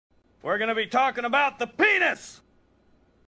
a premiere opacity masking test
i made this to learn masking in premiere but here it is i guess idk i've been messing around with a bit